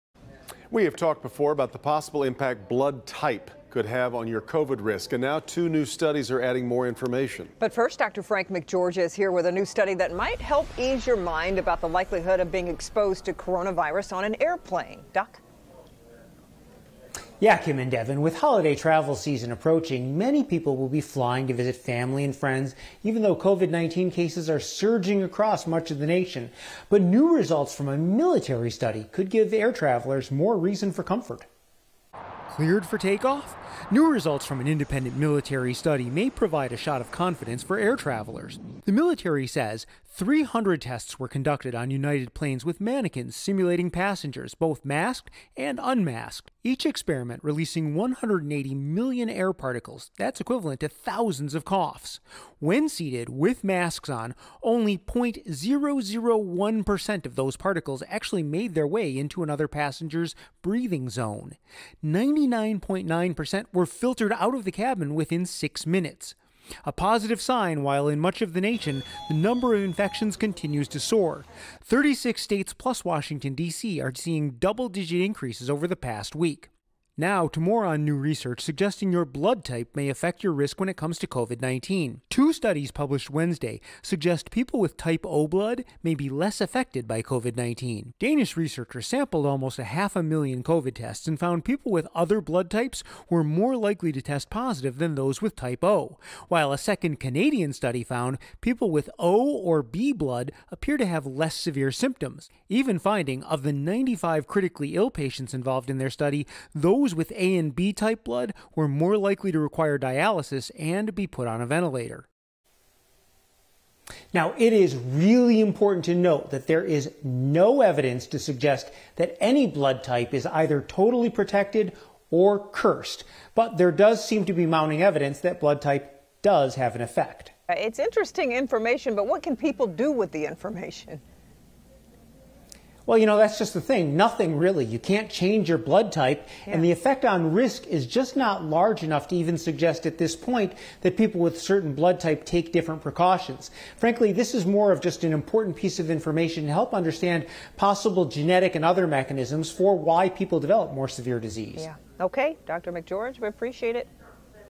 How likely are you to catch COVID-19 on an airplane? What impact does blood type have on the virus?
There are two new studies you should know about when it comes to the novel coronavirus (COVID-19).